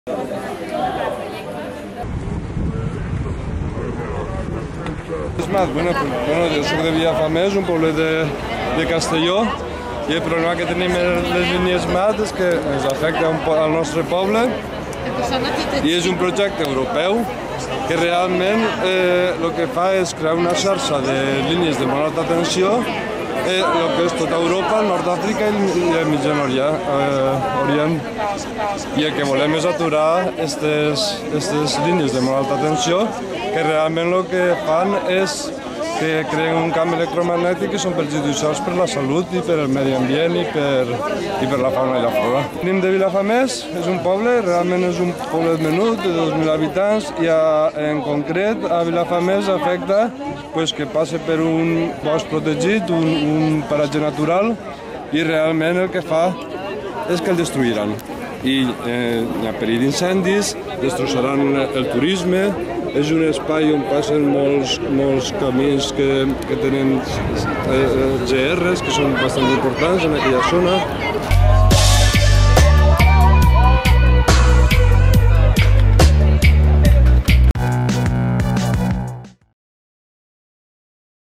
Volt3 (1): Denuncien la MAT davant de la Red Eléctrica de España
Barcelona, 29/10/2016.- Inici del "Volt 3: Davant la Impunitat Corporativa, Sobirania Popular". Activistes de diferents moviments denuncien els projectes de línies de Molt Alta Tensió davant la seu de Red Eléctrica Española, al Paral.lel de Barcelona.
El Volt3 és un moviment d´enxarxament de lluites pel territori que enguany recorrerà i denunciarà el projecte Castor, la pretroquímica de Tarragona i de Flix i la central nuclear de Vandellós. Més info